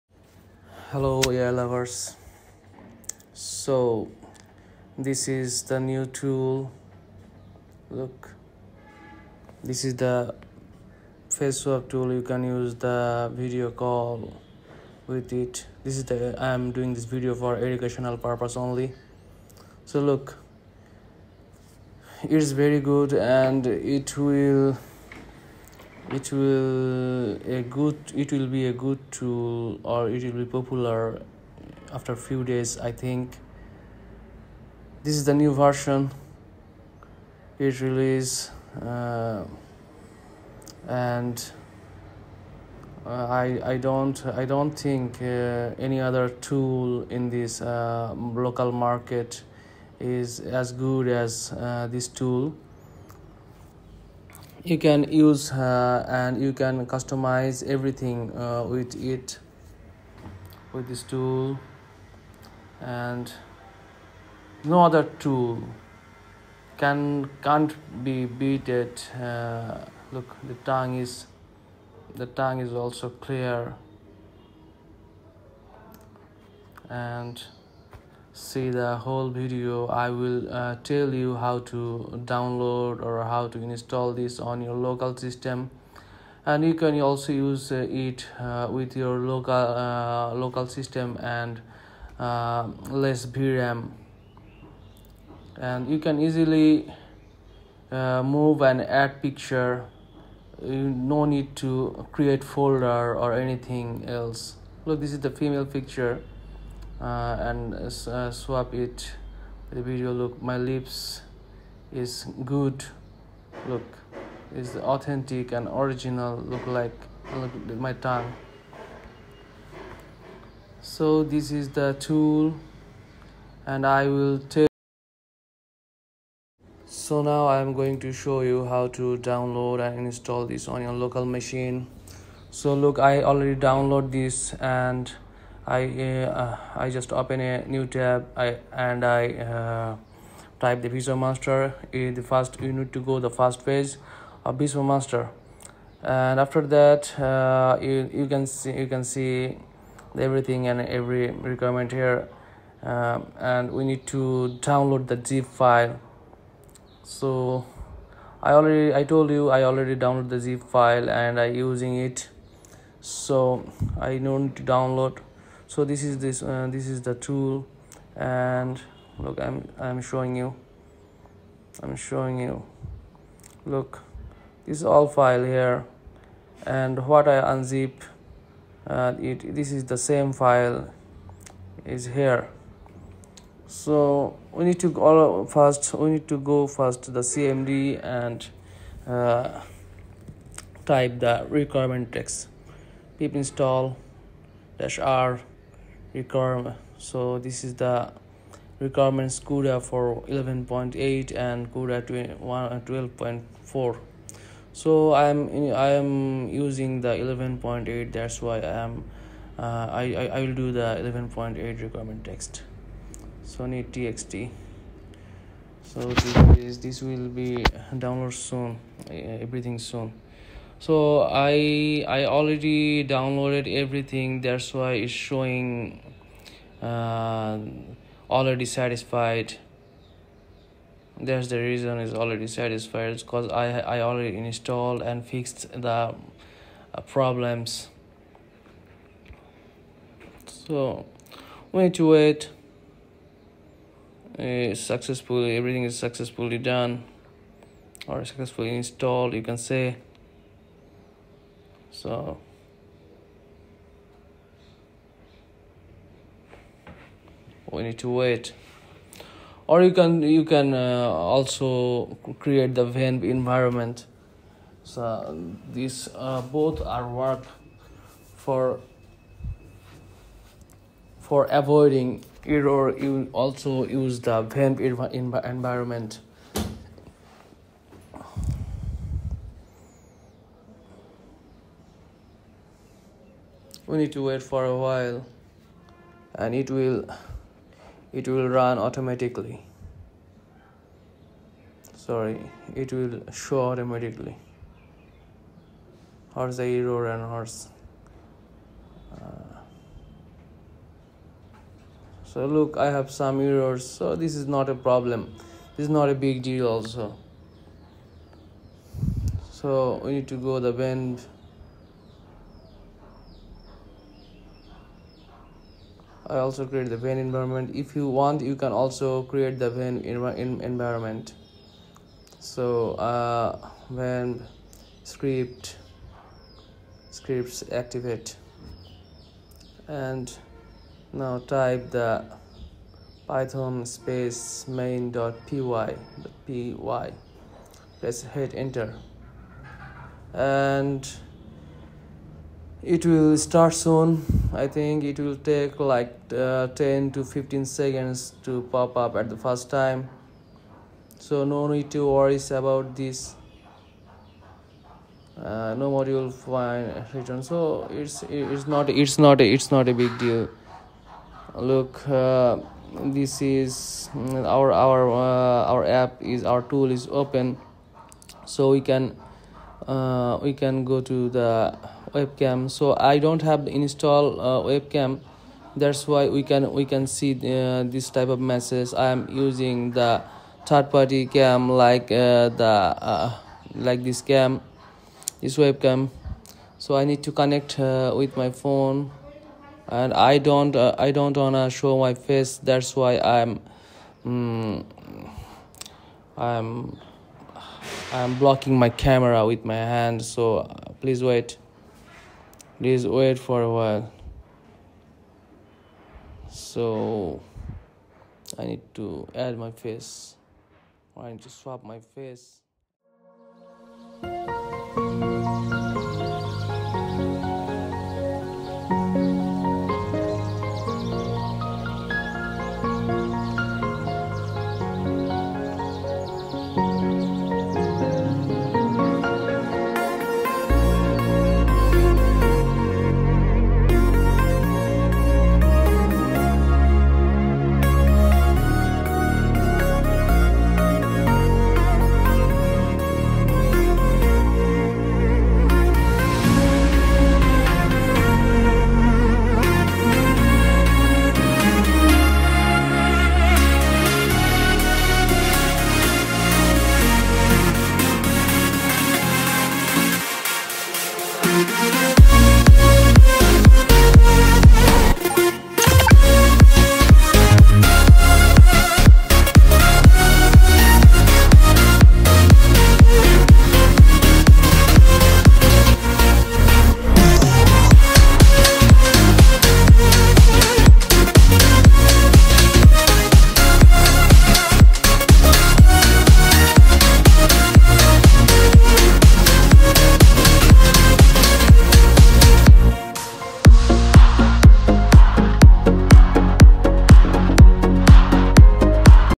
Visomaster 0.1.6 with Occlusions Install VisoMaster (Rope Live) Face Swap in 1-Click -Roop Unleashed
Greetings, AI enthusiasts This is the new tool and You can do a good output . At the first time i got this types of results. You can easily do the settings about your need . It’s better than roop deepfacelive #Deeplivecam deepfacecam2.0 This is dfm alternative. Now you no need to create the dfm models.

Portable file D-Drive (only approve for Buy me a cooffee )

If you have any question Please Comment OR If you need Extra care or help